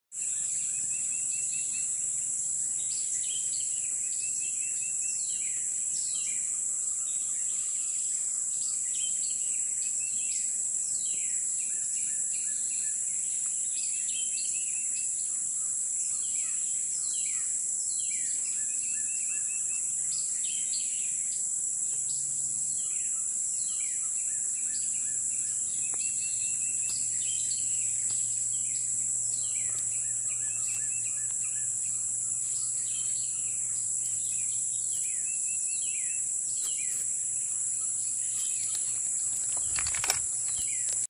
Close-up of Confused Eusarca (Eusarca confusaria)
This is a close-up of a moth, specifically Eusarca confusaria. Taken at Brazos Bend State Park.